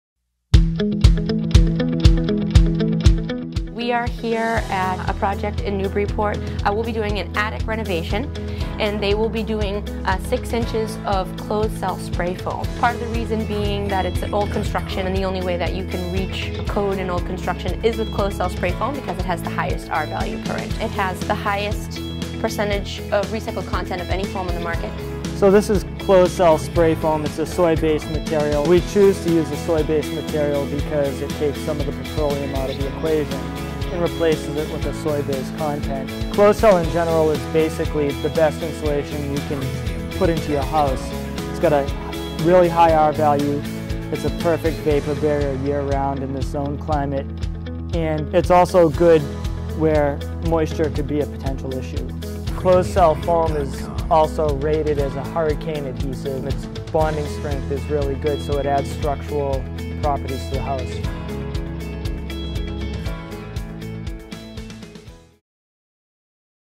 The Green Cocoon Closed Cell Spray Foam HD
Closed cell spray foam reduces excess waste and energy consumption in buildings by up to 50%. It also adds strength, R-value (7 per inch), and greater resistance to the leakage of air and water vapor. It is a “perfect vapor barrier” in both summer and in winter.

Closed cell spray foam works well with these types of projects:

Commercial
• New construction
• Existing energy saving upgrades
• Energy analysis & air sealing
• Moisture control
Industrial
• Commercial coolers / freezers
• Processing plants
• Production facilities
Residential
• New construction
• Existing home additions / remodels
• Energy savings upgrades
• Garages
• Out buildings